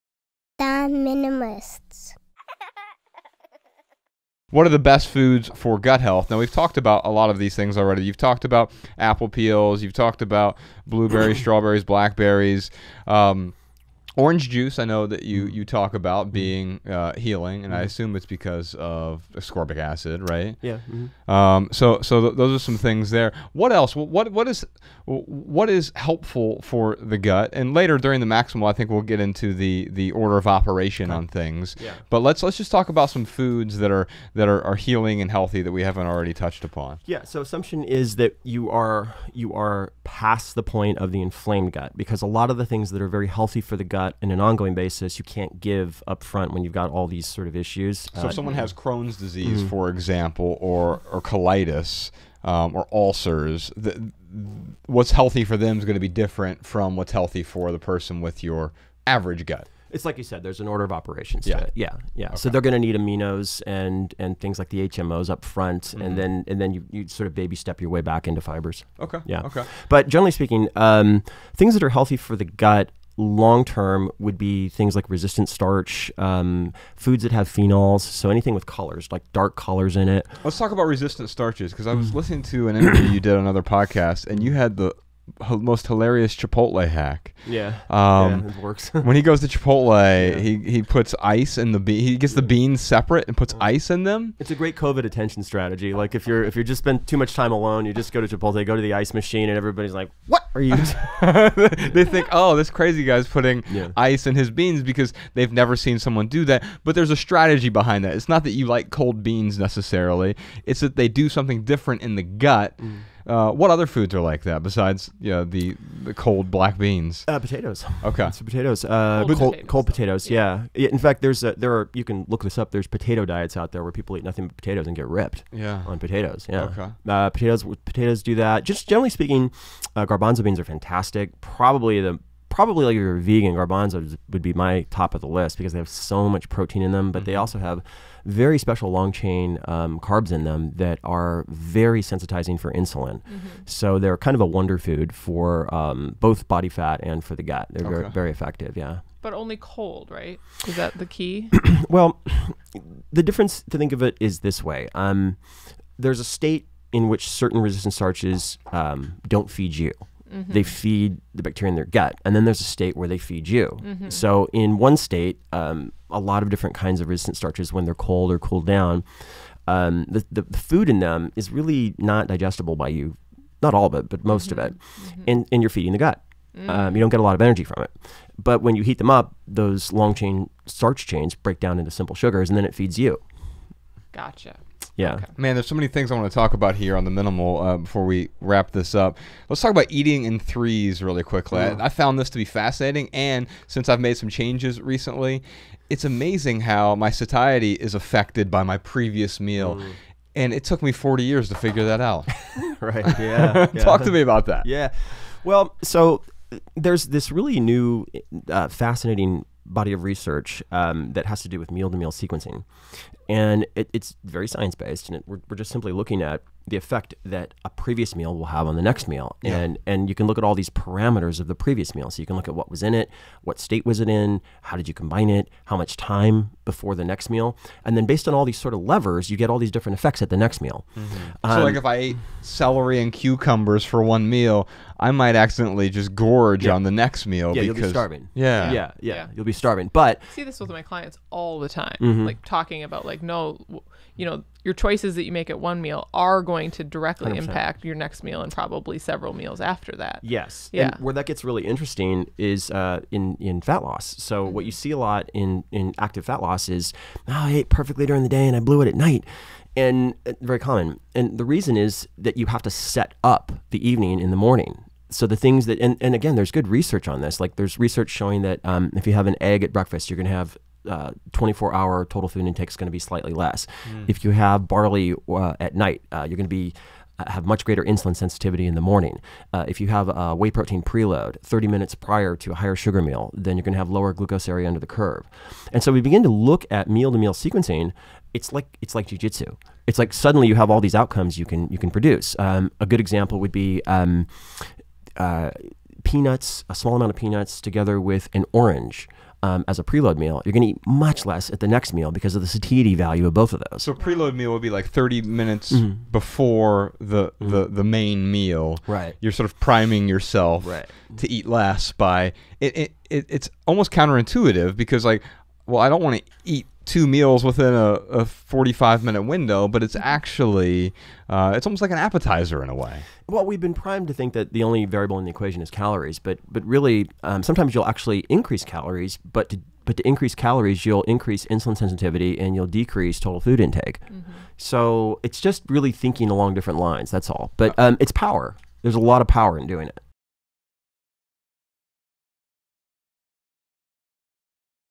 What is the best diet for a healthy gut?
Joshua and Rebecca discuss the most ideal foods for a healthy gut with author and VEEP Nutrition CEO Joel Greene.